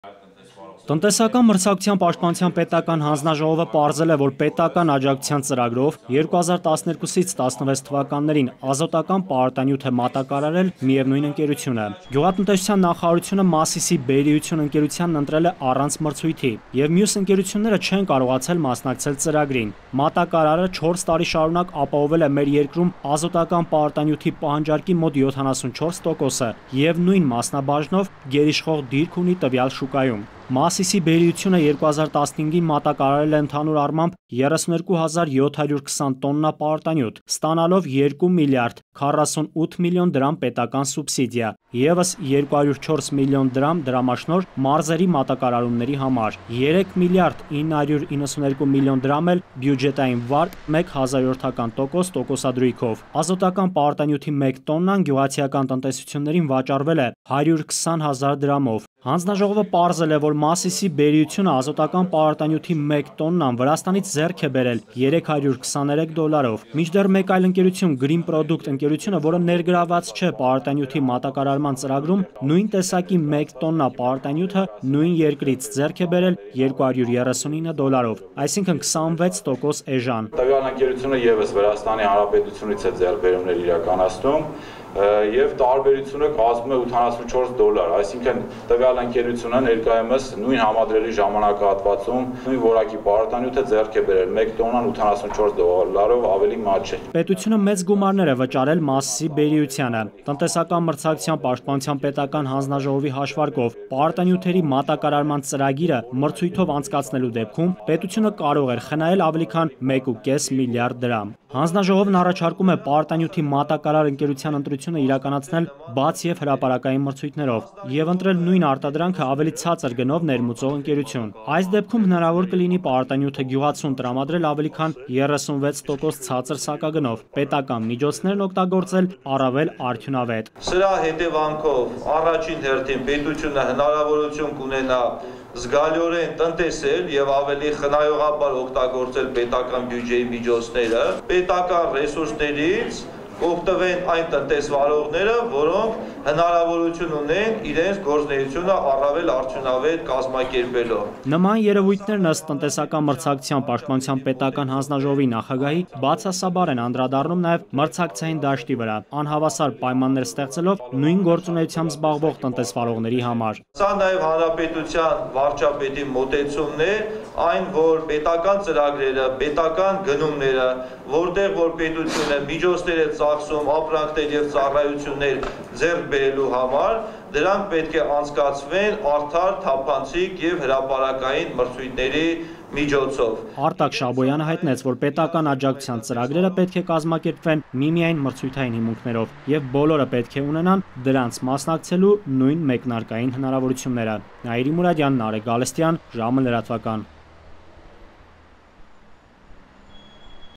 Պետական աջակցության ծրագրերը պետք է կազմակերպվեն միմիայն մրցութային հիմունքներով
Պարարտանյութերն առանց մրցույթի մատակարարելու պատճառով ՀՀ-ն կորցրել է տարեկան 1,5 միլիադ դրամ: